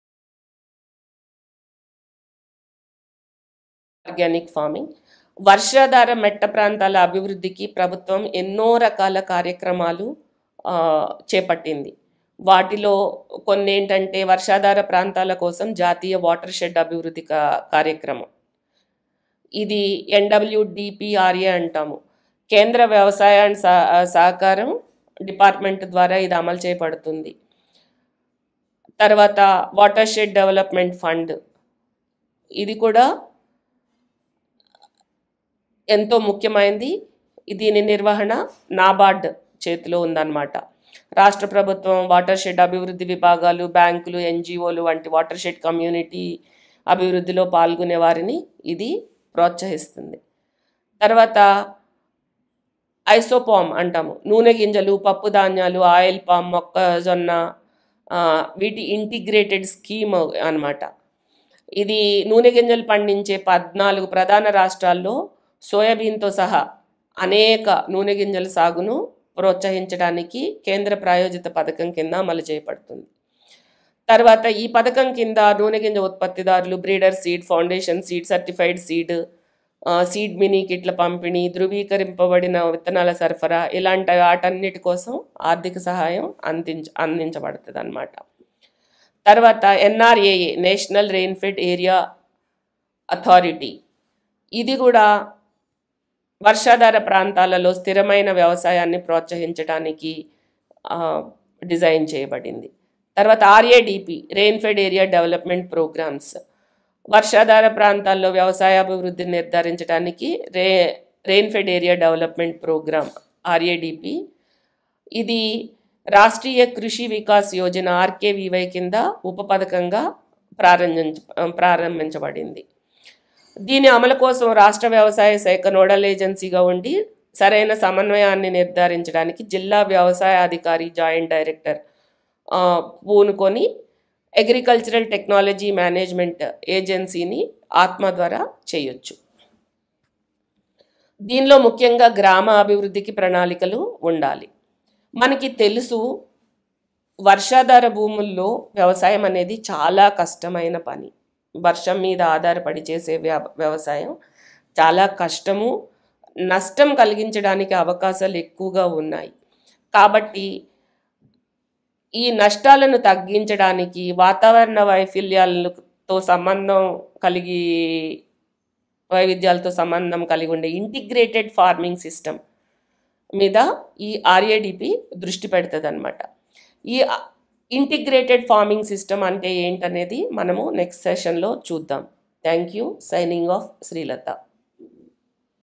వర్షాధార/ మెట్ట ప్రాంతాల అభివృద్ధికి ప్రభుత్వ కార్యక్రమాలు
NWDPRA (వర్షాధార ప్రాంతాల కోసం జాతీయ వాటర్‌షెడ్ అభివృద్ధి కార్యక్రమం): ఇది మాక్రో మేనేజ్మెంట్ ఆఫ్ అగ్రికల్చర్ (MMA) పథకం క్రిందకేంద్ర వ్యవసాయం & సహకారం డిపార్ట్మెంట్ ద్వారా అమలుచేయబడింది. ఈ పధకం XI పంచవర్ష ప్రణాళికలో సహజ వనరుల పరిరక్షణ, అభివృద్ధి,స్థిరమైన నిర్వహణ కోసం వర్షాధార ప్రాంతాలలో వాడటానికి సవరించబదిండి (FAO)  .
WDF (వాటర్‌షెడ్ డెవలప్‌మెంట్ ఫండ్): దీనిని 2000 సం. లో  కేంద్ర ప్రభుత్వం స్థాపించింది. మొత్తం కార్పస్ DAC మరియు NABARD సమానంగా సమకూర్చగా దాని నిర్వహణ నాబార్డ్‌తో ఉంటుంది. ఈ WDF రాష్ట్ర ప్రభుత్వం,  వాటర్‌షెడ్ అభివృద్ధి విభాగాలు, బ్యాంకులు, ఎన్జిఓలు వంటి వాటర్‌షెడ్ కమ్యూనిటీ అభివృద్ధిలో పాల్గొనేవారిని ప్రోత్సహిస్తుంది (నాబార్డ్).
ISOPOM (నూనెగింజలు, పప్పుధాన్యాలు, ఆయిల్ పామ్, మొక్కజొన్న యొక్క ఇంటిగ్రేటెడ్ స్కీమ్): ఇది నూనెగింజల పండించే 14 ప్రధాన రాష్ట్రాల్లో సోయాబీన్తో సహా నూనె గింజల సాగును ప్రోత్సహించడానికి కేంద్ర ప్రాయోజిత పథకం క్రింద అమలు చేయబడుతోంది. ఈ పథకం క్రింద నూనెగింజల ఉత్పత్తి దారులకు బ్రీడర్స్ సీడ్, ఫౌండేషన్ సీడ్, సర్టిఫైడ్ సీడ్ ఉత్పత్తి, సీడ్ మినీ కిట్ల పంపిణీకి,ధ్రువీకరించబడిన విత్తనాల సరఫరా చేయడానికి ఆర్థిక సహాయం అందించబడుతుంది.  పంట రక్షణ కొరకు రసాయనాలు కొనడానికి, రక్షణ పరికరాలు సమకూర్చుకోవడానికి, సూక్ష్మపోషకాల సరఫరా, మెరుగైన వ్యవసాయ పనిముట్ల సరఫరా, స్ప్రింక్లర్ సెట్ల పంపిణీ, నీటిని తీసుకువెళ్ళే పైపుల పంపిణీ కొరకు సబ్సిడీ కూడా ఇస్తారు. సేంద్రియ ఉత్పత్తిని ప్రోత్సహించడానికి అపరాలకు రైజోబియం కల్చర్ సరఫరా చేయడానికి, ఫాస్ఫేట్ ద్రావణీకరణ  బ్యాక్టీరియా, జిప్సం, పైరైట్, లైమింగ్, డోలమైట్ వంటి సేంద్రియ ఇన్పుట్ల పంపిణీకి ఆర్థిక సహాయం అందించబడింది. వీటిని ఉపయోగించడానికి శిక్షణ కూడా ఇవ్వబడుతుంది (డిపిడి).
NRAA (నేషనల్ రెయిన్‌ఫెడ్ ఏరియా అథారిటీ): వర్షాధార ప్రాంతాలలో స్థిరమైన వ్యవసాయాన్ని ప్రోత్సహించి వర్షాధార రైతుల జీవనోపాధిని మెరుగుపరచడానికి NRAA స్థాపించబడింది. వర్షాధార వ్యవసాయాన్ని ఆర్థికంగా లాభసాటిగా మార్చడమే NRAA యొక్క ముఖ్య ఉద్దేశం.  వ్యవసాయ సమాజం యొక్క జీవనోపాధిని మెరుగుపరచడానికి వ్యవసాయ-పర్యావరణ శాస్త్రం, వ్యవసాయ-జీవవైవిధ్యం, స్థిరత్వం ఆధారంగా సంక్షేమాన్ని కలిగించడం దీని ముఖ్య ఉద్దేశం  (NRAA).
RADP (రెయిన్‌ఫెడ్ ఏరియా డెవలప్‌మెంట్ ప్రోగ్రామ్): వర్షాధార ప్రాంతాల్లో వ్యవసాయ వృద్ధిని నిర్ధారించడానికి “రెయిన్‌ఫెడ్ ఏరియా డెవలప్‌మెంట్ ప్రోగ్రామ్ (RADP)” 2011-12 సంవత్సరంలో రాష్ట్రీయ కృషి వికాస్ యోజన (RKVY) క్రింద ఉప పథకంగా ప్రారంభించబడింది.  దీని ప్రధాన దృష్టి చిన్న, సన్నకారు రైతులపై ఉంచి వ్యవసాయ కార్యకలాపాల పూర్తి ప్యాకేజీని అందించడం ద్వారా వారి వ్యవసాయ రాబడిని పెంచడం, వారి జీవన నాణ్యతను మెరుగుపరచడం లక్ష్యంగా పెట్టుకుంది.  దీని అమలు కోసం రాష్ట్ర వ్యవసాయ శాఖ నోడల్ ఏజెన్సీగా ఉండి  సరైన సమన్వయాన్ని నిర్ధారించడానికి జిల్లా వ్యవసాయ అధికారి / జాయింట్ డైరెక్టర్ పూనుకుని అగ్రికల్చరల్ టెక్నాలజీ మేనేజ్‌మెంట్ ఏజెన్సీని (ATMA) జోడించవచ్చు. ప్రాజెక్టు ప్రతిపాదనలలో గ్రామ అభివృద్ధి ప్రణాళికలు ఉండాలి. ఉత్పాదకతను పెంచడానికి, నష్టాలను తగ్గించడానికి వాతావరణ వైవిధ్యాలతో సంబంధం కలిగి ఉండే ఇంటిగ్రేటెడ్ ఫార్మింగ్ సిస్టమ్ పై  RADP దృష్టి పెడుతుంది (AGRICOOP).